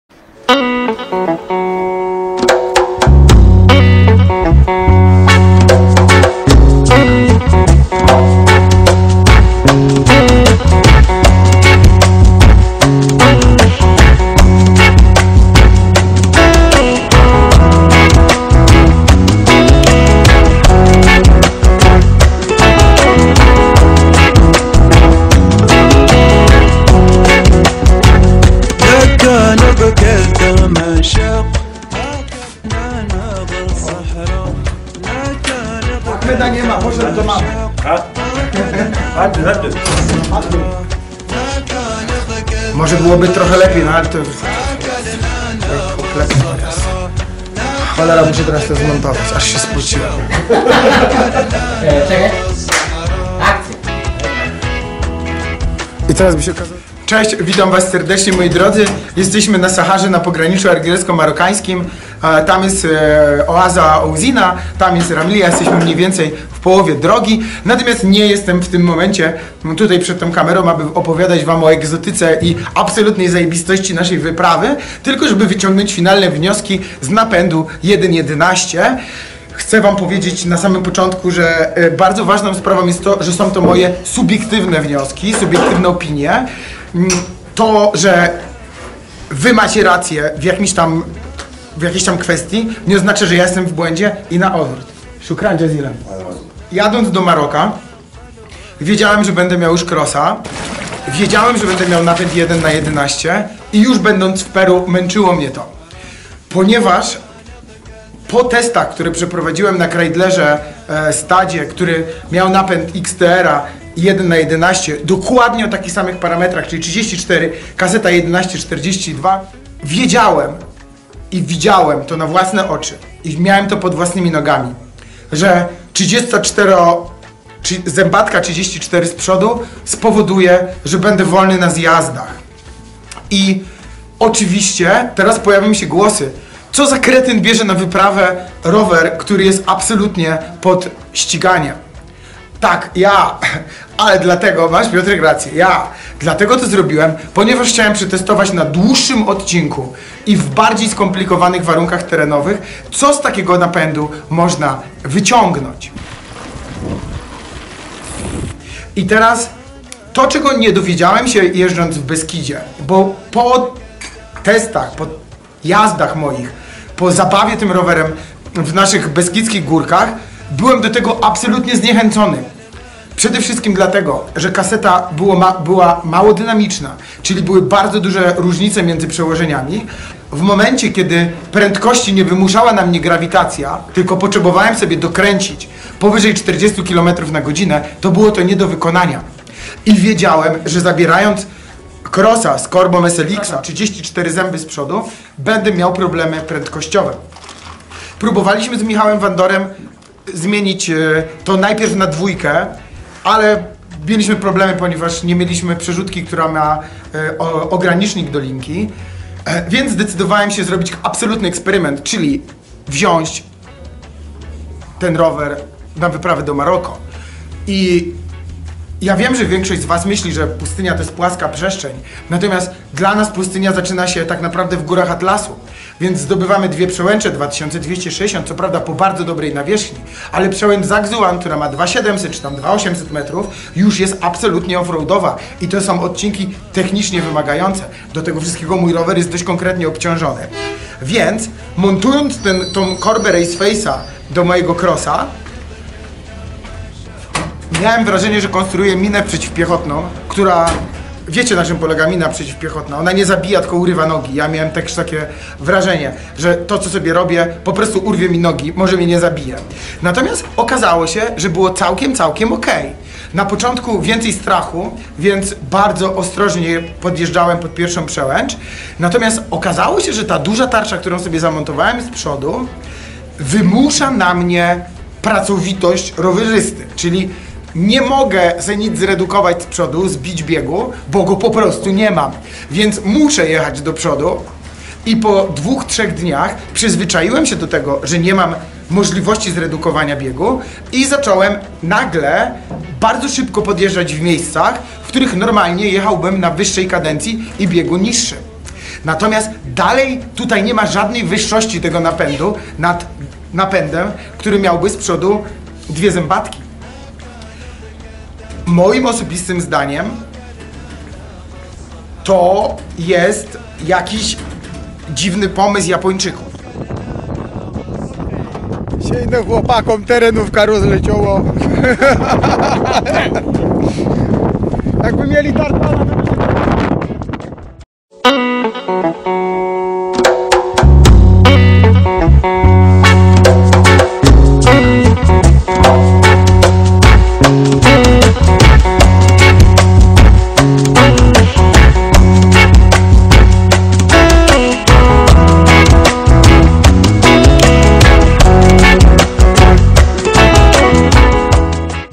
Napęd 1x11 Kross B8 dalsze wnioski cz. 1 (Sahara)
Porządna recenzja i przedstawienie wszystkich za i przeciw napędu 1x11 wymaga bardzo uczciwego podejścia do tematu. Zdecydowałem że filmik ci podzielę na kilka części. Po przejechaniu kilku tysięcy kilometrów jestem gotowy na przedstawienia wam mojej finalnej opinii.

Zapraszam:)